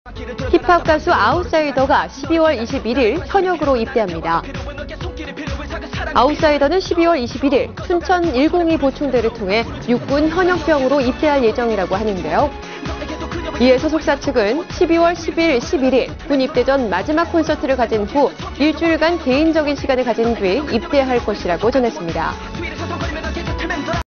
[music] Outsider Enlisted (아웃사이더, 12월 21일 현역 입대)
힙합 가수 아웃사이더가 12월 21일 현역으로 입대합니다.
아웃사이더는 12월 21일 춘천 102보충대를 통해 육군 현역병으로 입대할 예정이라고 하는데요.
이에 소속사측은 "12월 10일, 11일 군 입대 전 마지막 콘서트를 가진 후 일주일간 개인적인 시간을 가진 후 입대할 것"이라고 전했습니다.